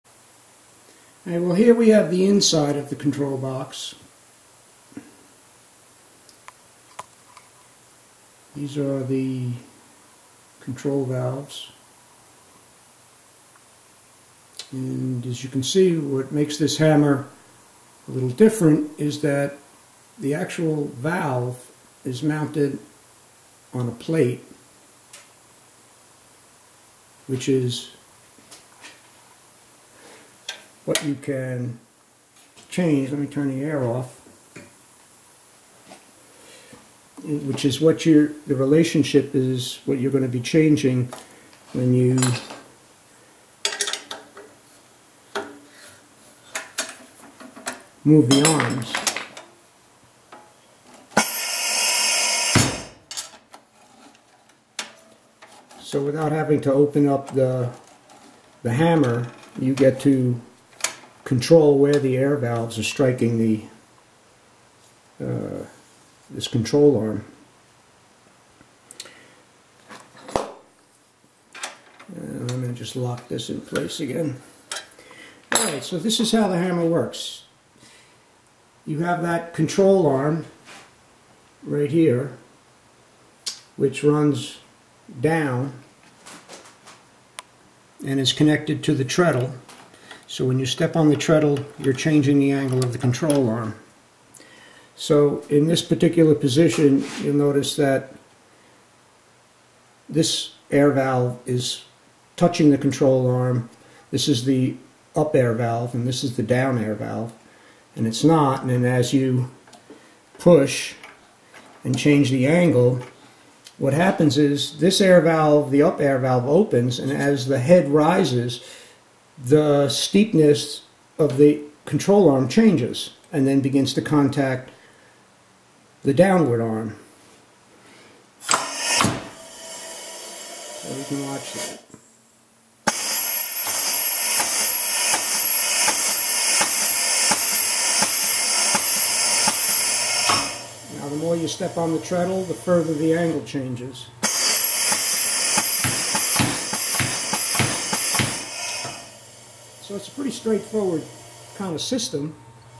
Hammer control.avi
Pneumatic valve control for forging hammer modeled after the original Bull forging hammer.

A non-liner timing that gives ultimate control of pneumatic cylinders used in forging hammers.